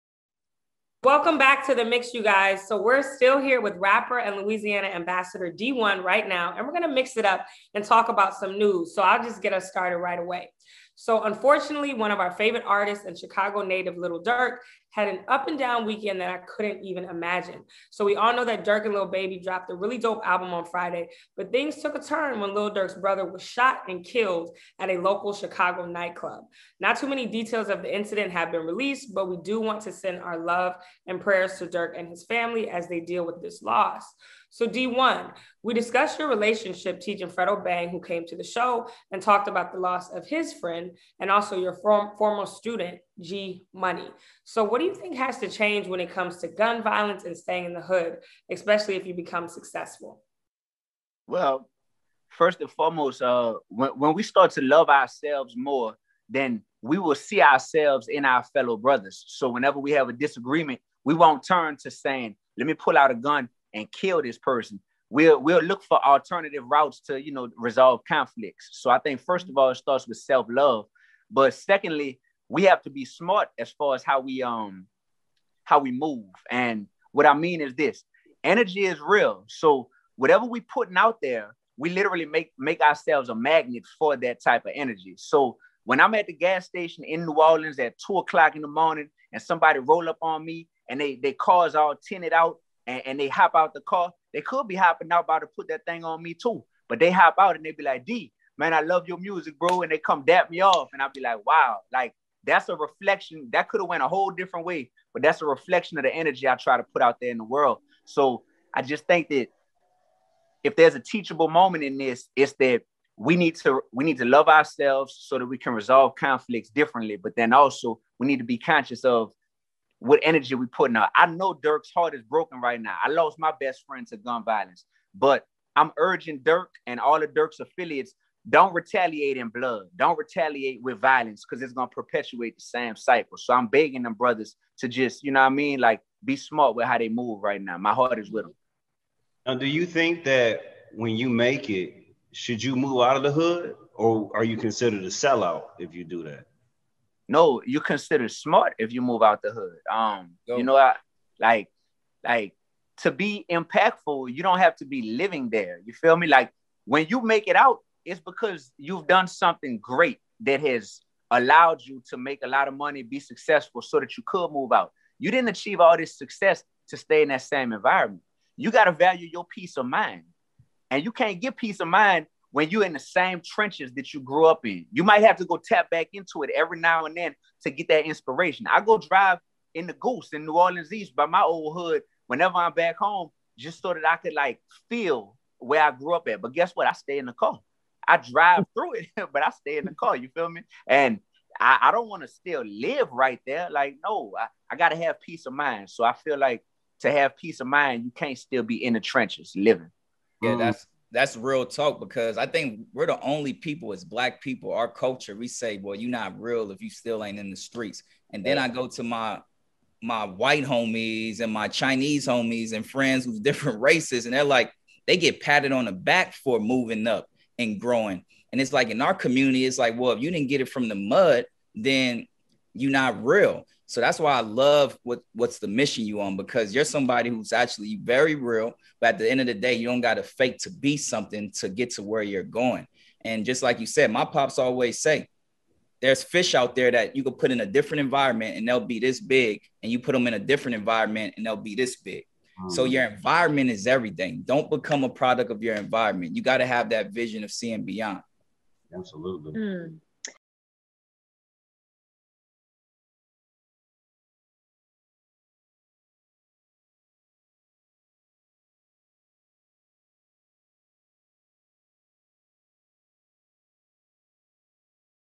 Rapper Dee-1 on Resolving Conflict and Gun Violence | The Mix
The Mix hosts sit down with Rapper Dee-1 to talk about how the black community needs to be smart about resolving conflict reduce gun violence in black neighborhoods.